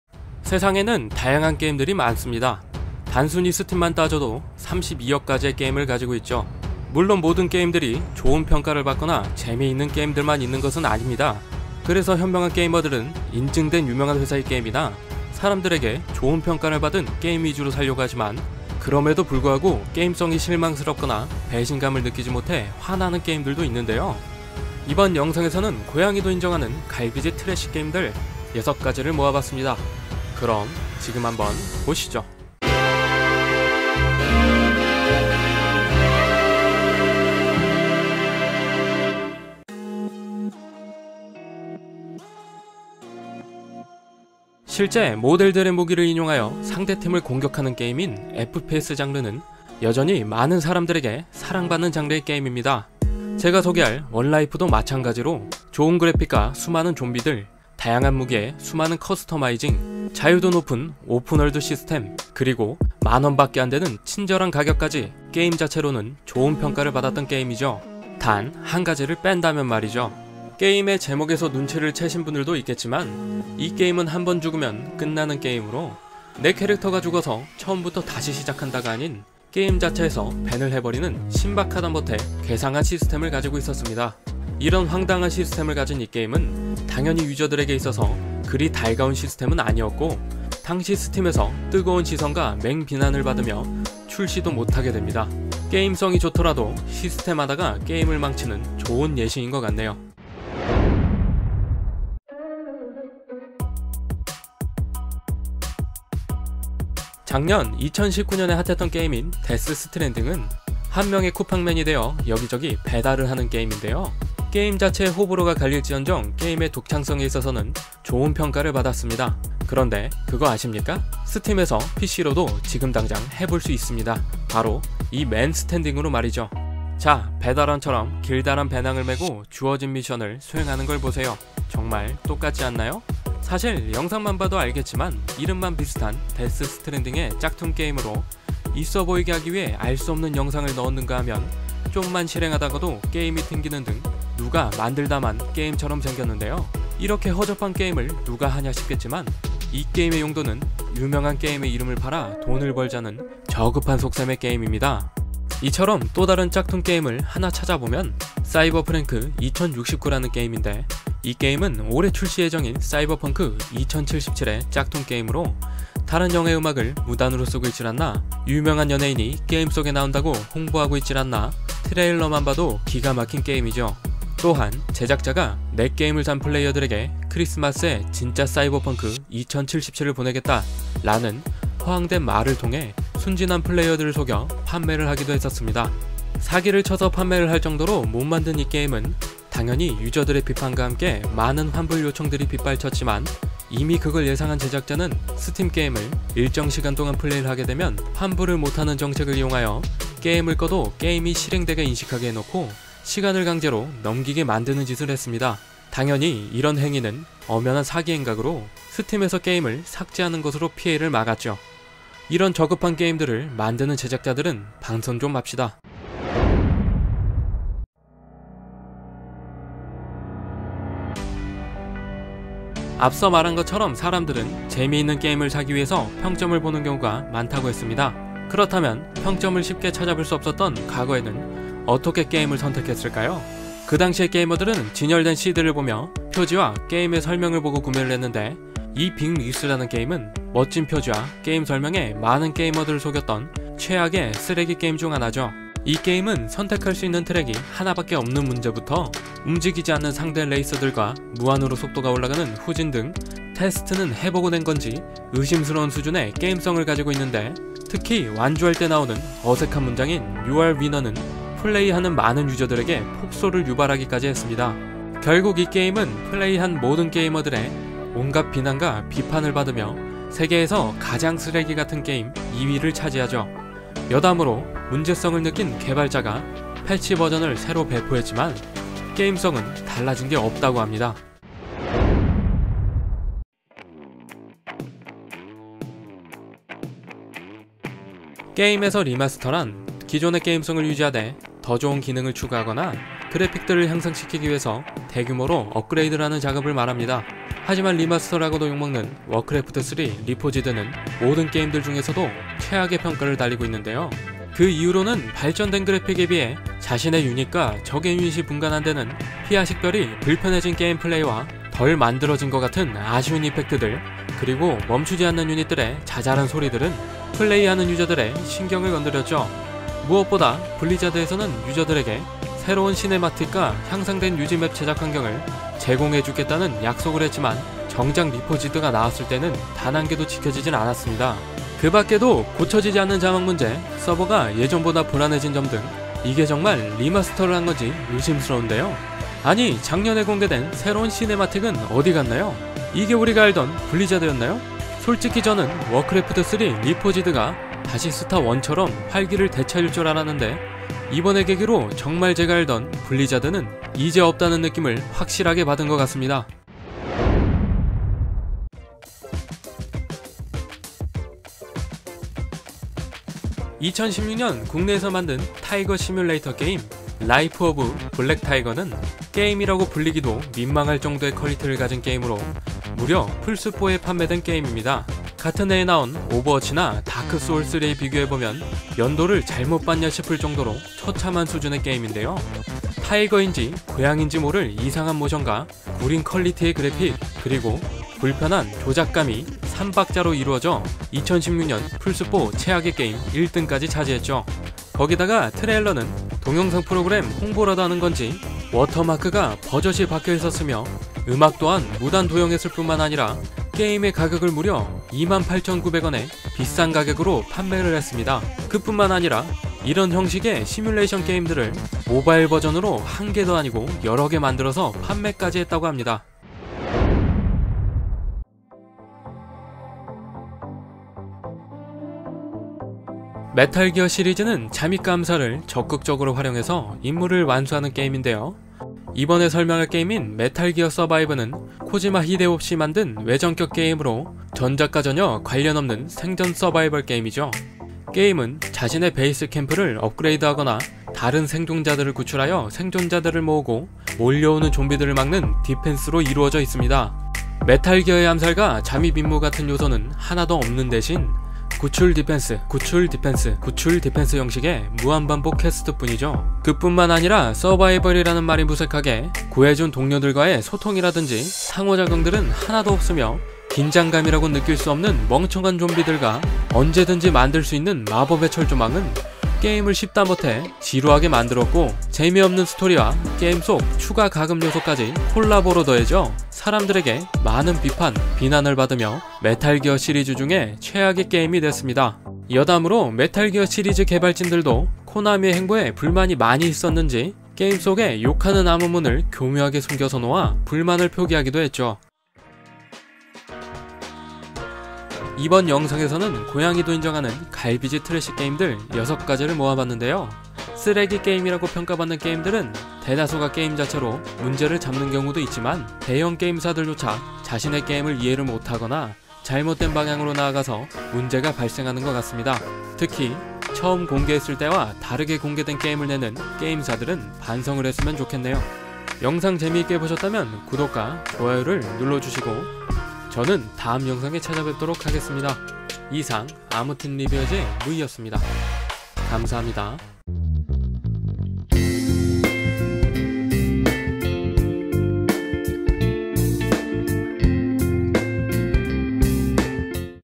고양이도 인정하는 갈비쥐 트레쉬 게임들 TOP6ㅣ아무튼리뷰어즈
안녕하세요
아무튼리뷰어즈의 므이 입니다!!
오늘은 고양이도 인정하는 갈비쥐 트레쉬 게임들 여섯가지를 모아보았습니다
저희는 다음주에 또 다른 주제로  돌아오겠습니다
감사합니다!

이 영상자막에는 티켓몬스터에서 제공한 몬소리체글꼴이 적용되어 있습니다.(tmon몬소리 black)